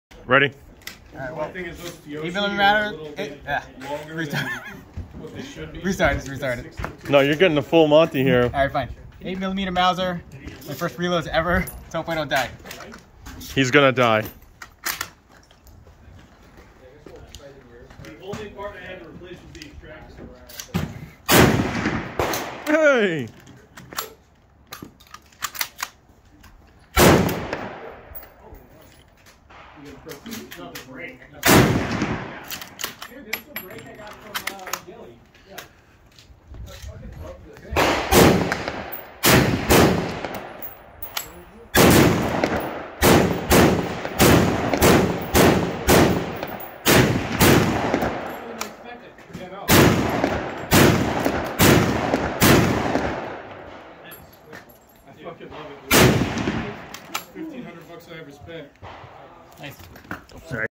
Shooting my M48A Yugoslavian Mauser
This is my Zastava M48A Yugoslavian Mauser. Here I am shooting my own reloads for the very first time (8mm Mauser).